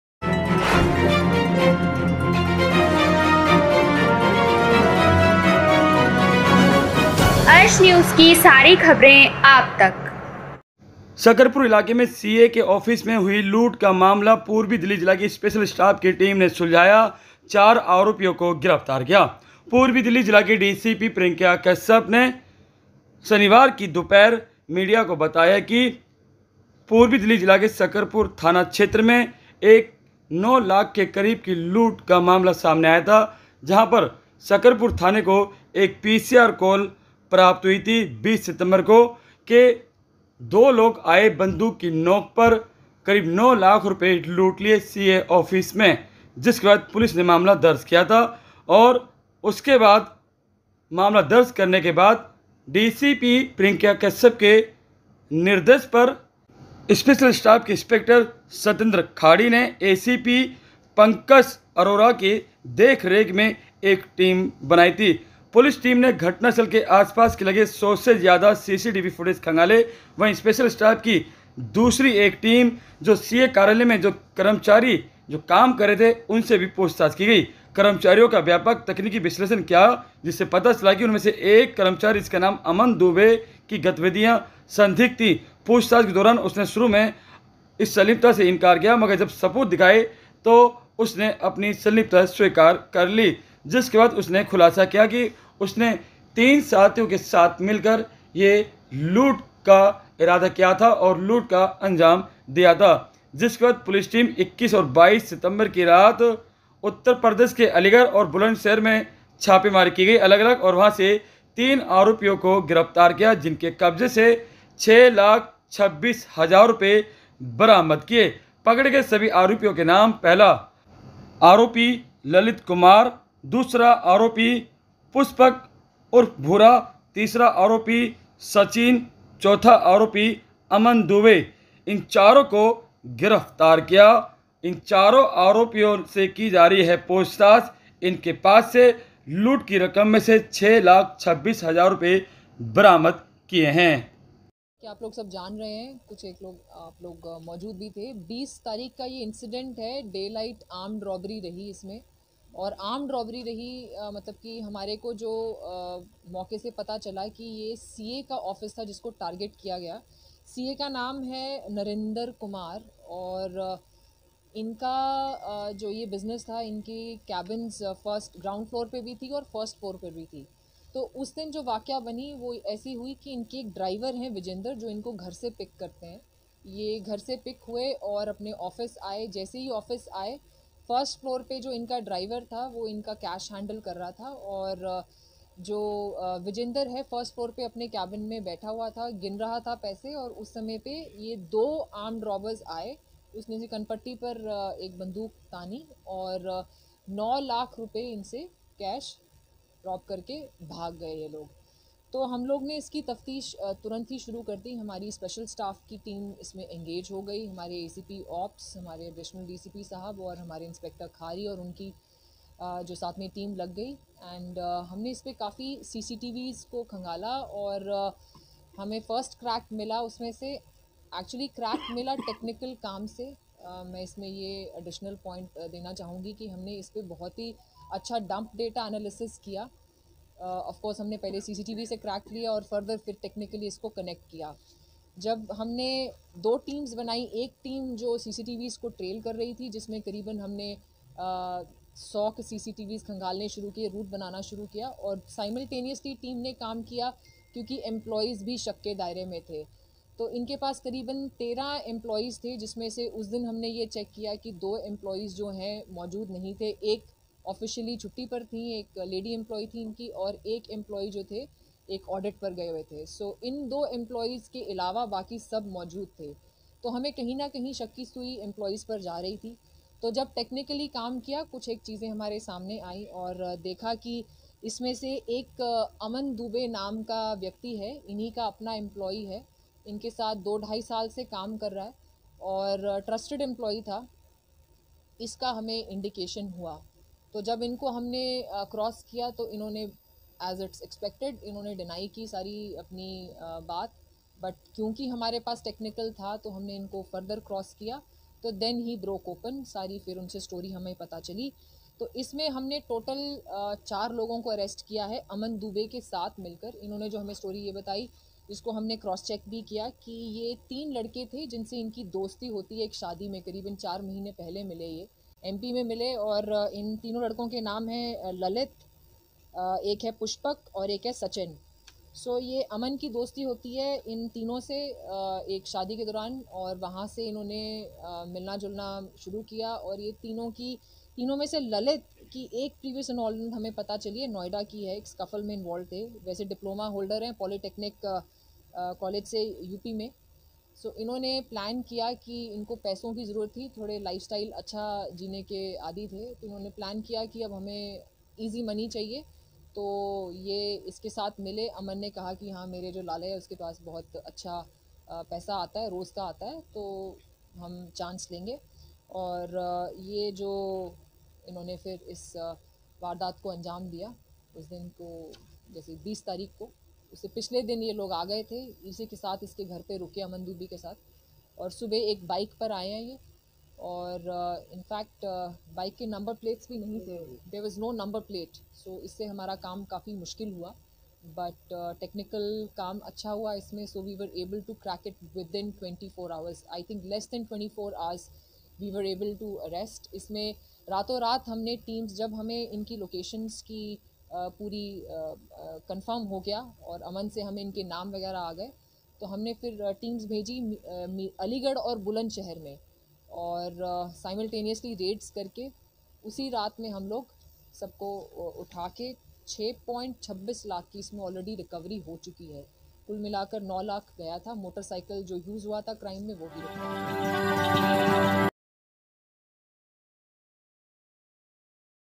शकरपुर में सीए ऑफिस से हुई लूट का मामला सुलझाया स्पेशल स्टाफ की टीम ने4 आरोपियों को गिरफ्तार किया
शकरपुर इलाके में सीए ऑफिस से हुई लूट का मामला सुलझाया पूर्वी दिल्ली जिला की टीम ने चार आरोपियों को गिरफ्तार कियाnews